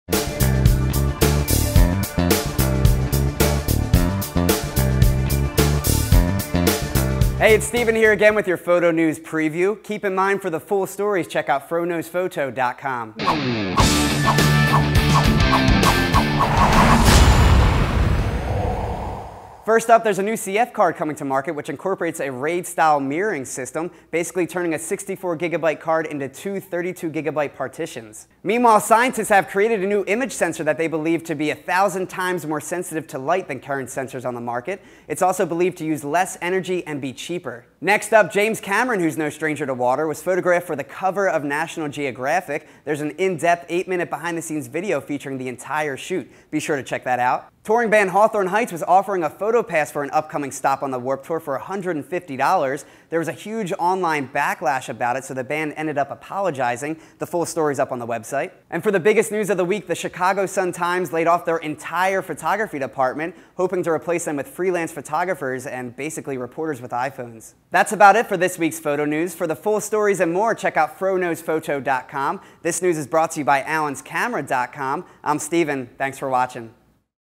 FroKnowsPhoto Photo News Preview of the WEEK 6/9/13
‎Click Here for the FULL News Stories.

Please take note that the FroKnowsPhoto NEWS video is a "PREVIEW" of the weeks major stories.  If a story interests you and you would like to read more about it please click the link above.

This week a touring band offers their fans a chance to "photograph" them for $150, which quickly faces backlash.

For the first time a CF card allows you to store files via a raid set up which at first I thought was a good idea but after thinking about it its really not.

The Chicago Sun Times decides to layoff their entire photo staff in favor of reporters with Iphones.

James Camron poses for an underwater photo shoot with National Geographics and you can see the behind the scens video over on the site.